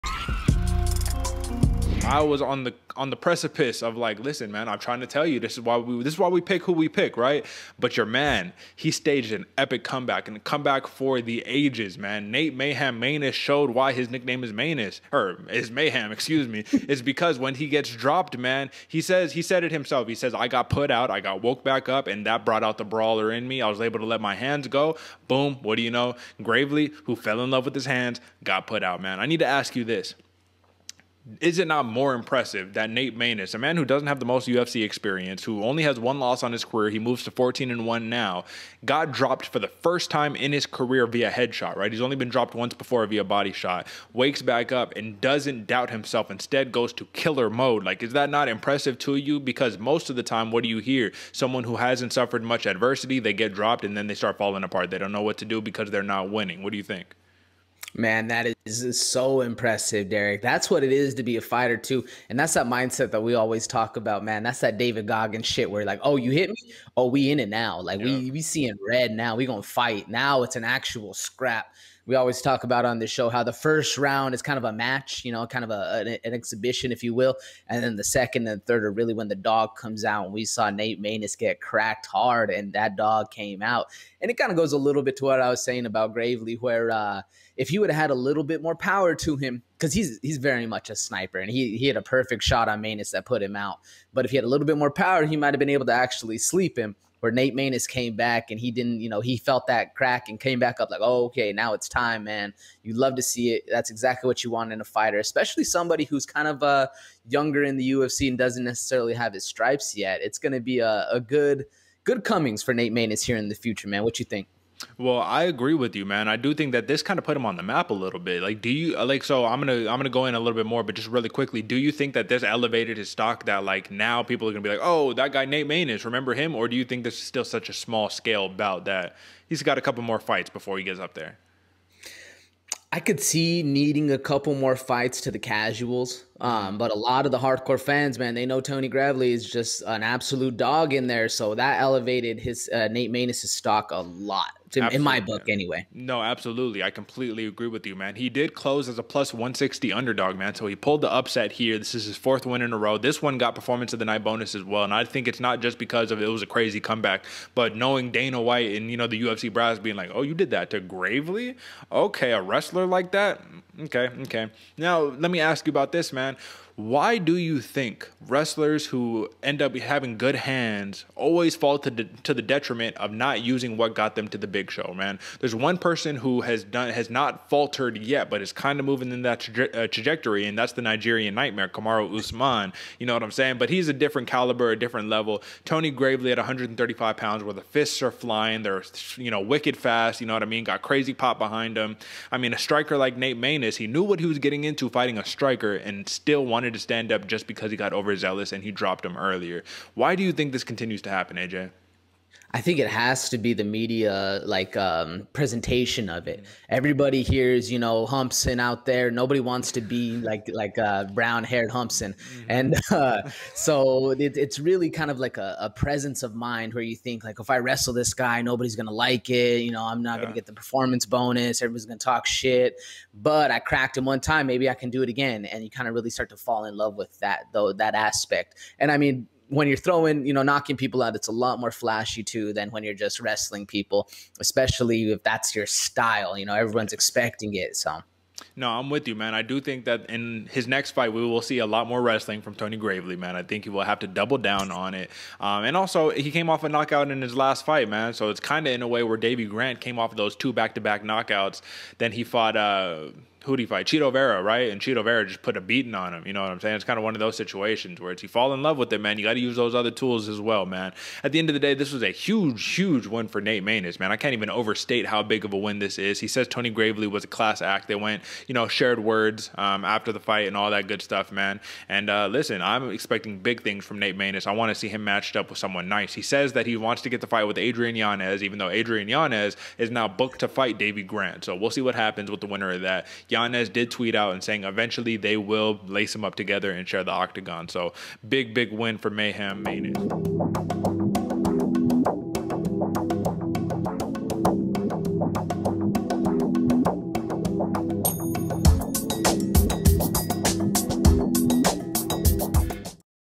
NATE MANESS OVERCOMES ADVERSITY TO WIN VIA KO | UFCVEGAS37 | Bloody Water Podcast
This clip is taken from Bloody Water Podcast Ep. #80 | UFC Vegas 37 Post-Show